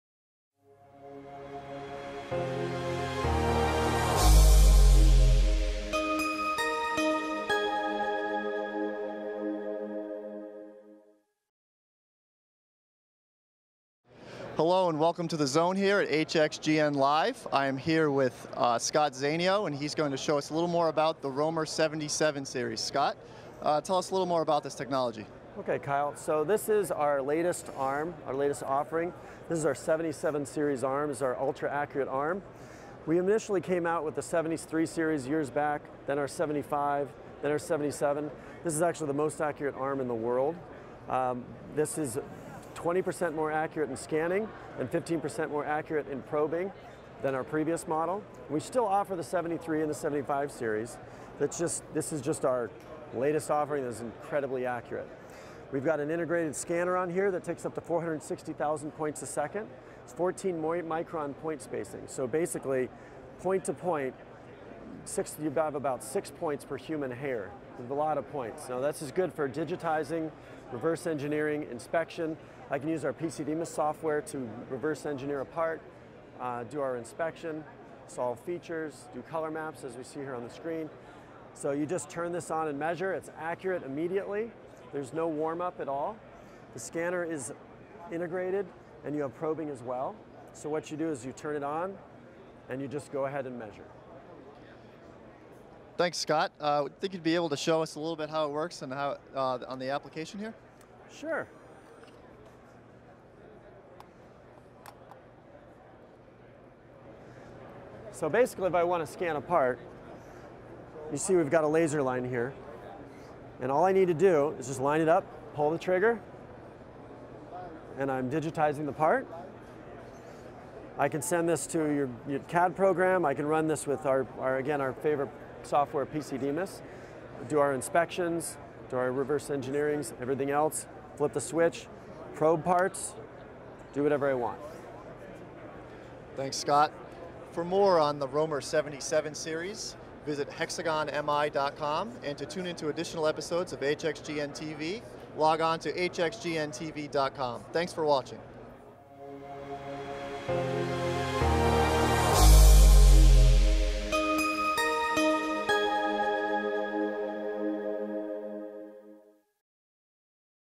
Absolute Quality: ROMER Absolute Arm 7725 SI (TV534)
Join Gene Daniell to learn about a first in the world of portable measuring arms: The ROMER Absolute Arm features absolute encoders and is therefore the first measuring arm which does not require referencing before measurement. When the arm is turned on, it's ready to go.